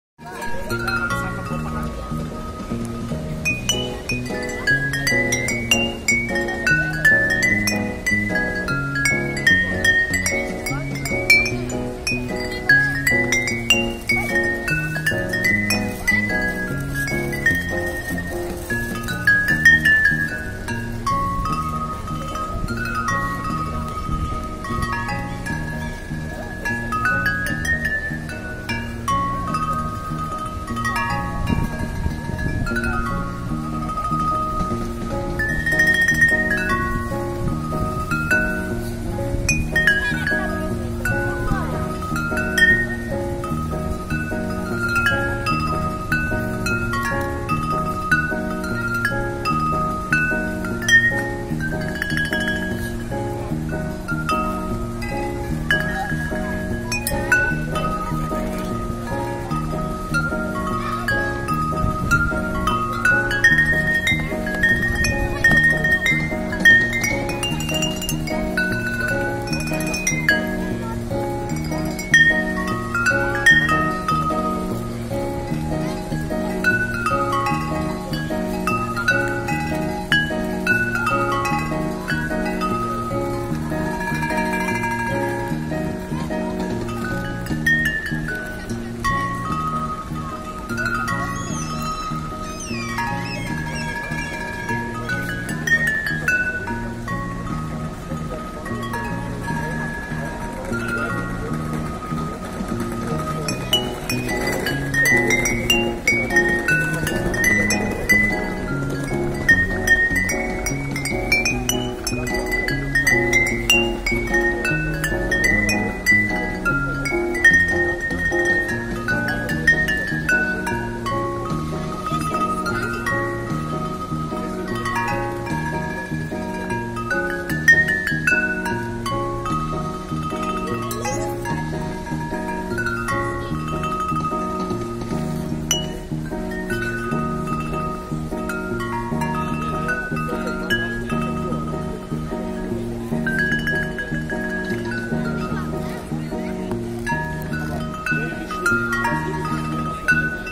Fedor grigorev bottle pianist - street music. Playing on Bottles in Bergen Norway. Gra na butelkach
Yesterday when i was Walking in Bergen City Center i saw the guy playing on bottles filled with water. Amazing bottle piano ;) His name is Fedor grigorev from Russia ;)